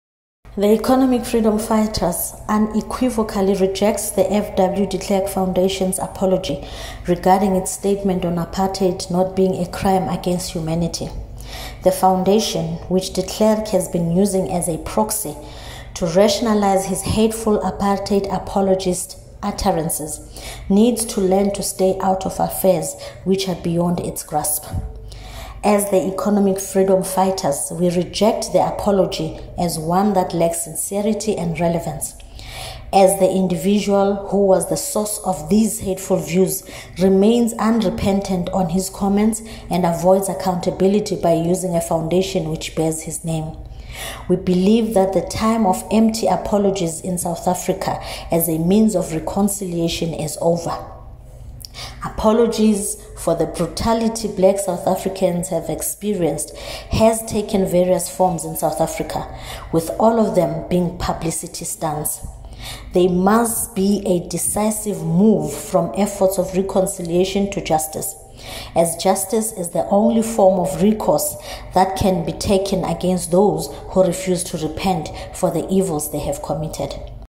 EFF REJECT DE KLERK’S APARTHEID APOLOGY
The EFF has rejected the FW de Klerk Foundation's apology regarding its statement on apartheid not being a crime against humanity. The party wanted De Klerk to be stripped of his Nobel Peace Prize and any privileges he got as a former head of state. The EFF also called for him to face the might of the law. This is the latest development following the EFF's call for the removal of De Klerk from Parliament ahead of President Cyril Ramaphosa's State of the Nation Address last week